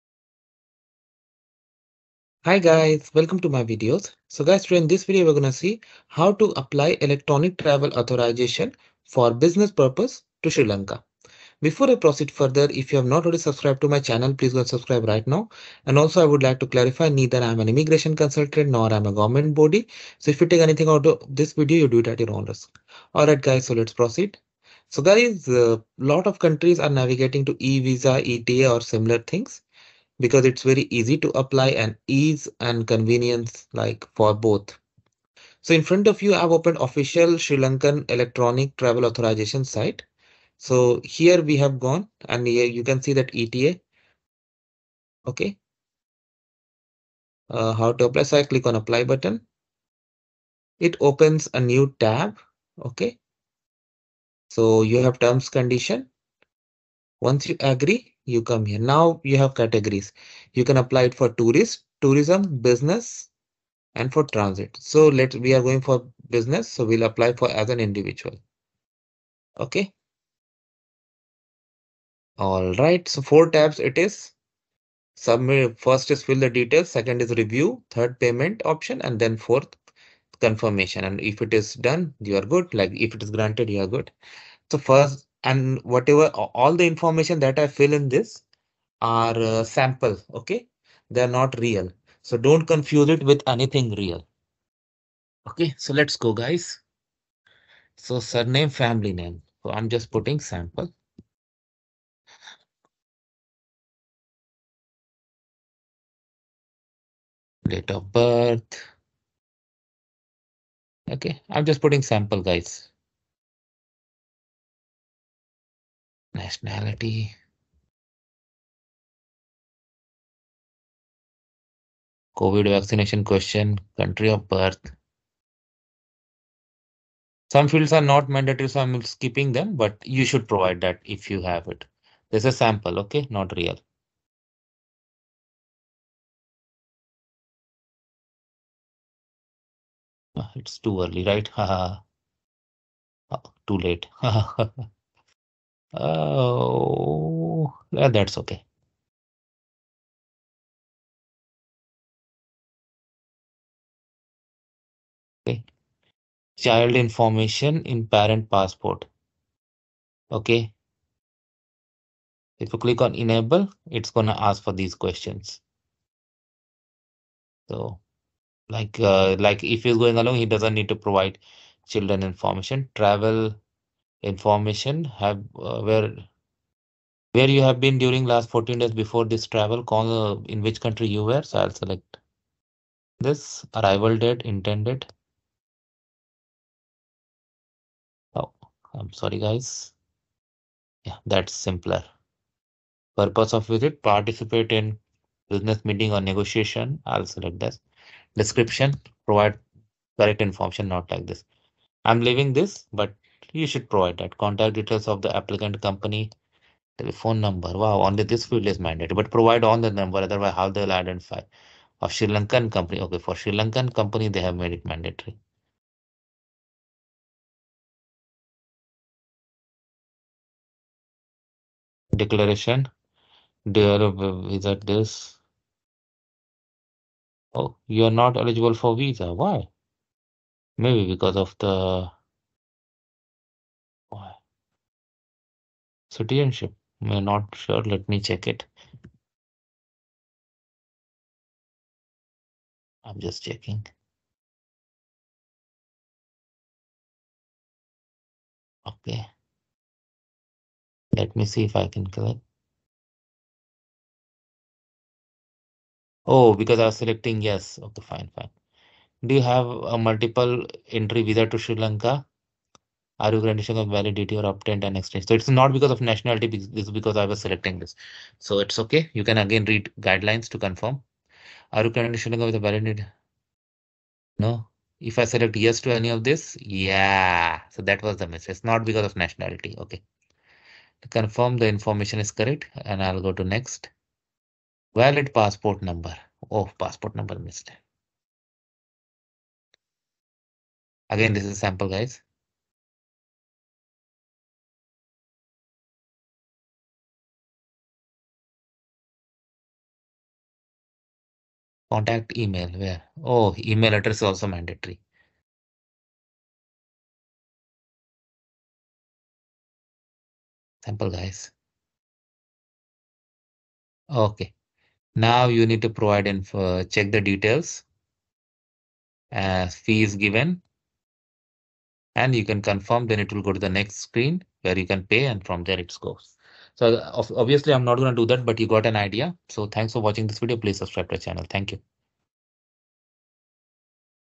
How To Apply ETA (Electronic Travel Authorization ) Business Visa To Travel To Sri Lanka
Watch how How To Apply ETA (Electronic Travel Authorization ) To Travel To Sri Lanka Full Information. Watch it full and let me know what you think about it